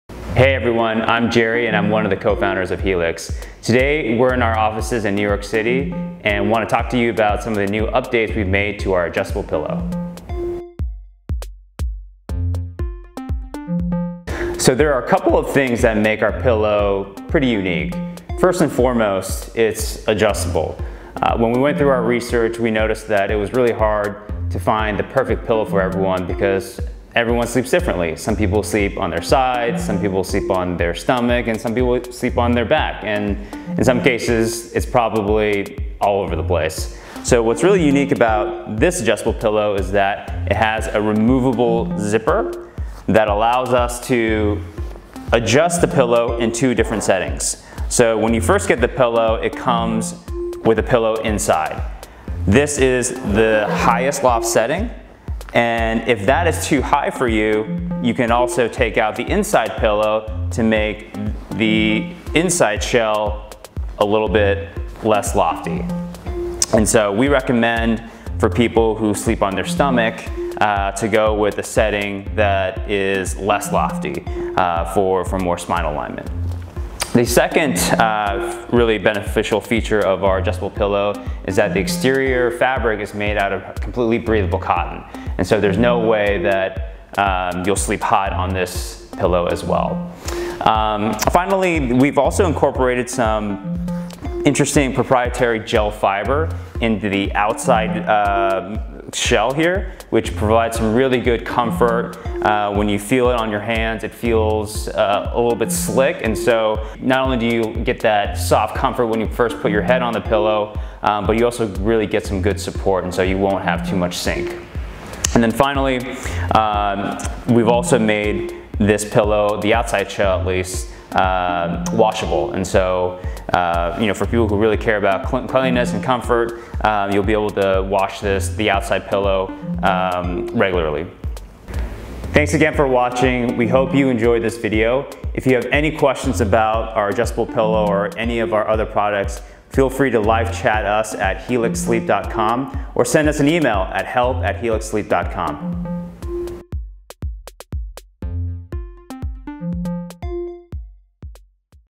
Helix Adjustable Pillow
New Age Adjustable Design
Each pillow has a removable support insert layer so you can adjust the height of the pillow and find your most comfortable loft.

Built For All Sleepers
Adjustable for all sleeping positions and body types - perfect whether your like soft or firm.

Breathable Materials
The Adjustable Pillow features breathable cotton and gel microfiber fill to make sure you don’t get too hot at night.